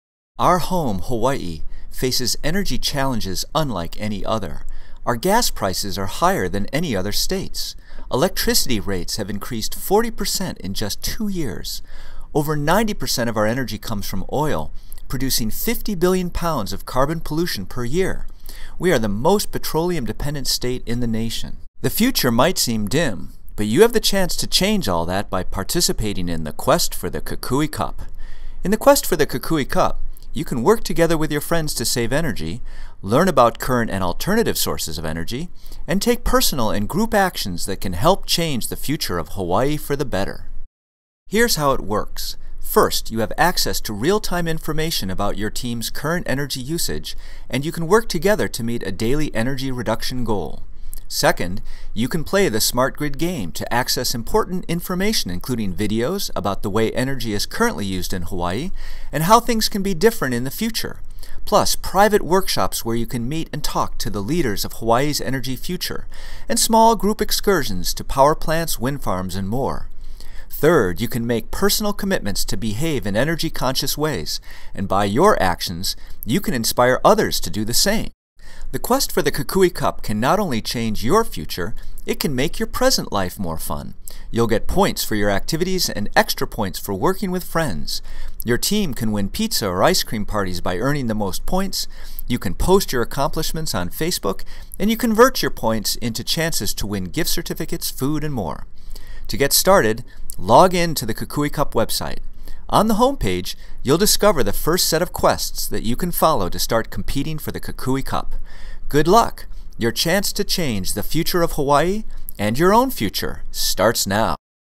Introduction to the Kukui Cup
Learn about the 2012 Kukui Cup, an advanced energy challenge.